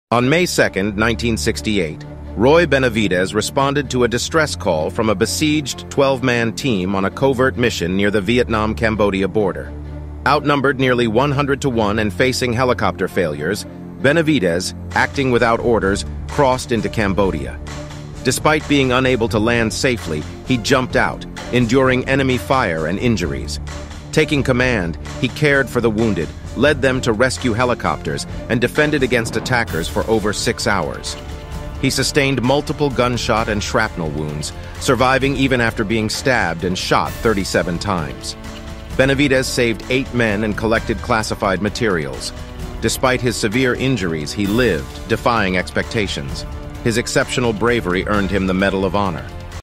🎖️ Green Beret Awarded Medal Of Honor
On May 2, 1968, Roy Benavidez responded to a distress call from a besieged 12-man team on a covert mission near the Vietnam-Cambodia border. Outnumbered nearly 100-to-one and facing helicopter failures, Benavidez, acting without orders, crossed into Cambodia.

Despite being unable to land safely, he jumped out, enduring enemy fire and injuries. Taking command, he cared for the wounded, led them to rescue helicopters, and defended against attackers for over six hours.

He sustained multiple gunshot and shrapnel wounds, surviving even after being stabbed and shot 37 times. Benavidez saved eight men and collected classified materials.

Despite his severe injuries, he lived, defying expectations. His exceptional bravery earned him the Medal of Honor.